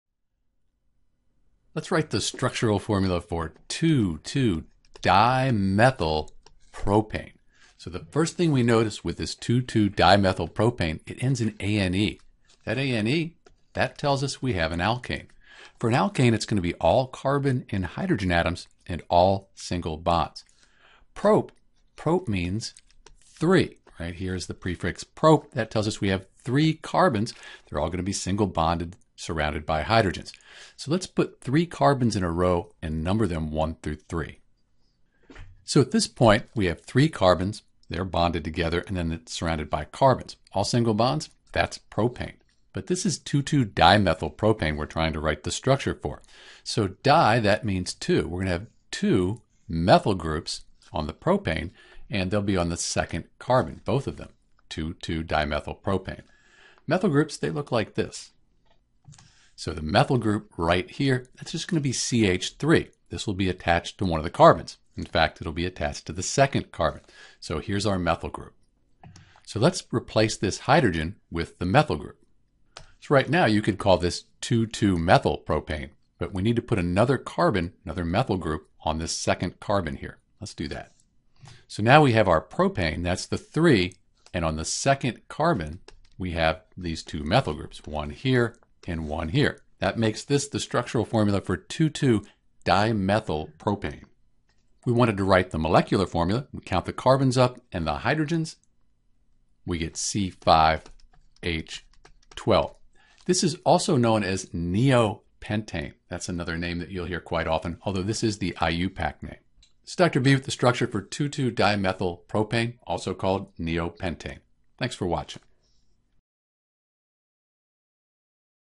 Structural Formula for 2,2-Dimethypropane
To write the structure for organic molecule 2,2-Dimethypropane (also called neopentane) we’ll start by writing propane.  Propane is an alkane with three carbon atoms connected with single bonds.  Next, we’ll number the Carbon atoms one through three.

Since it is 2,2-Dimethypropane we’ll but two methyl groups (-CH3) on Carbon atom number two.  This gives us the correct structure for  2,2-Dimethypropane.

We can also write the structure using straight lines without the element symbols, which is a sort of molecular structure shorthand.  Finally, we could represent the structure with molecular models to provide further information about the molecular geometry.